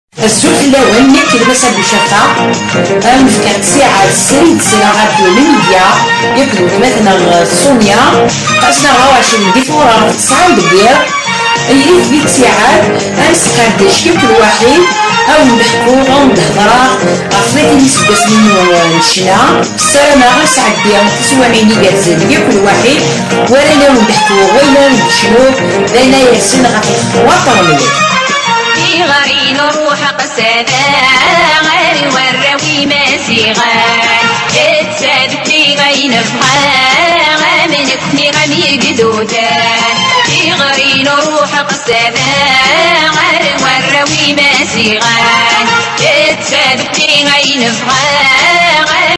Massa Bouchafa
Massa Bouchafa invitee de Sonia Ait-Ahmed sur Radio Numydia le 24 Fevrier 2020 @ 21h00